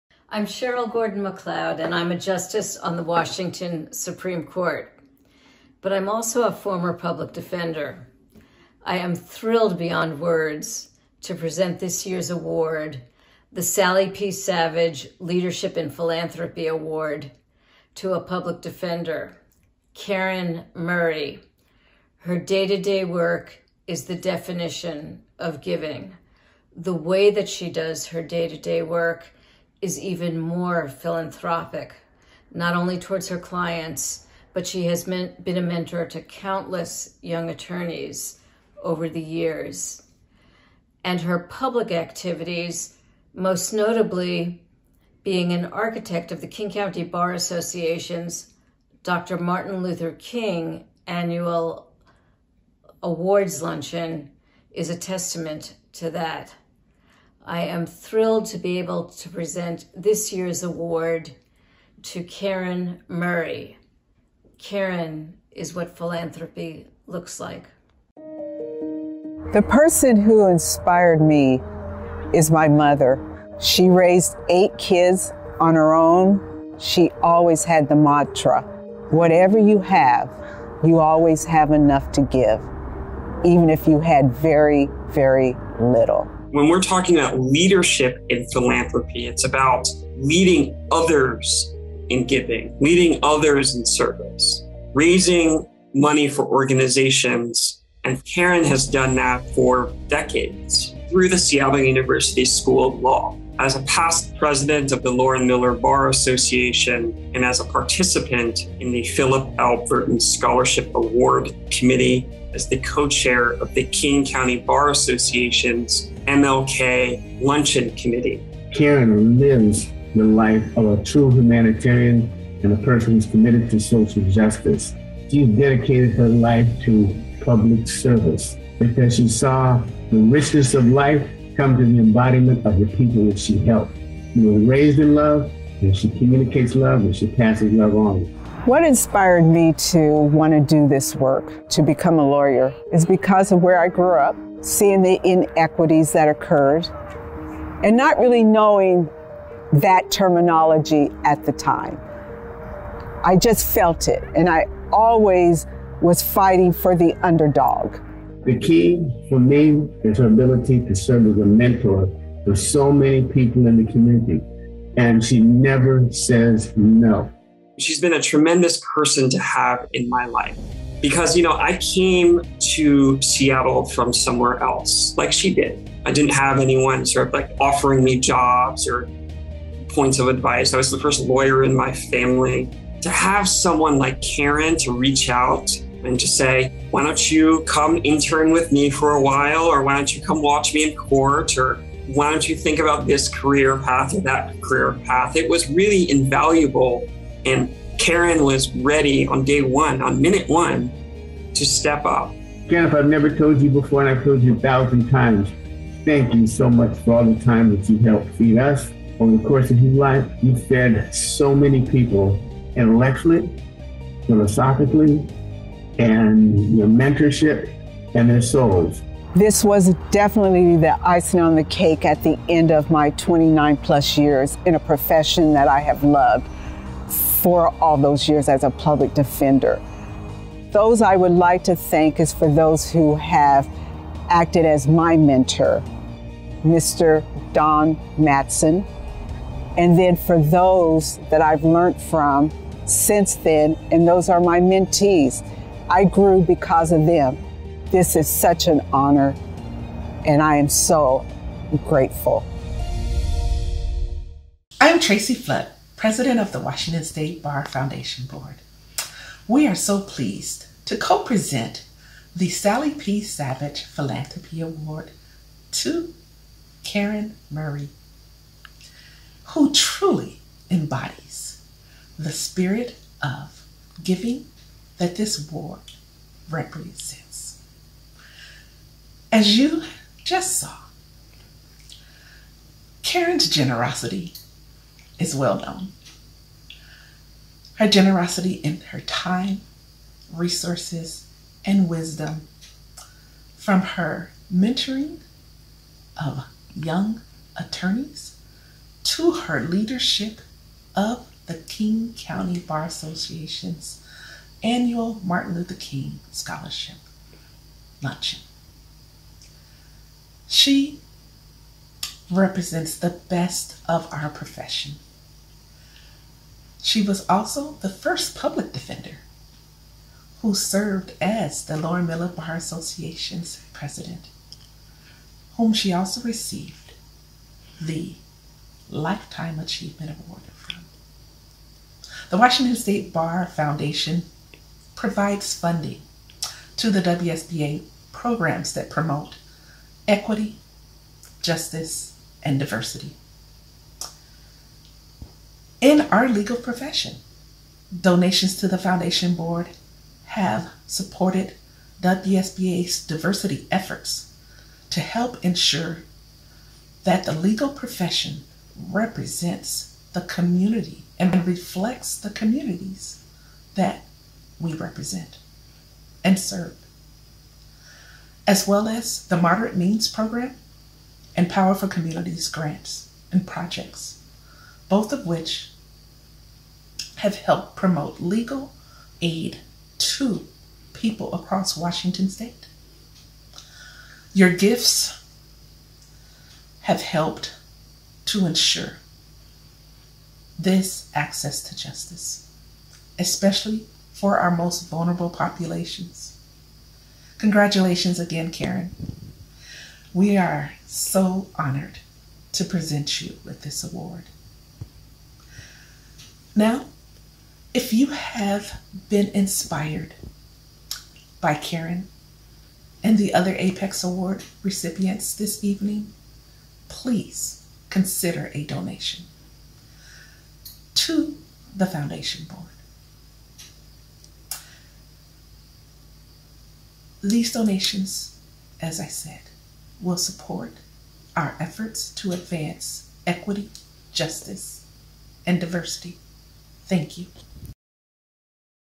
The 2021 APEX Awards: Sally P. Savage Leadership in Philanthropy Award Recipient Karen W. Murray
The 2021 APEX Awards webcast was recorded live on Monday, Dec. 13. The annual awards celebrate the incredible accomplishments of Washington’s legal luminaries.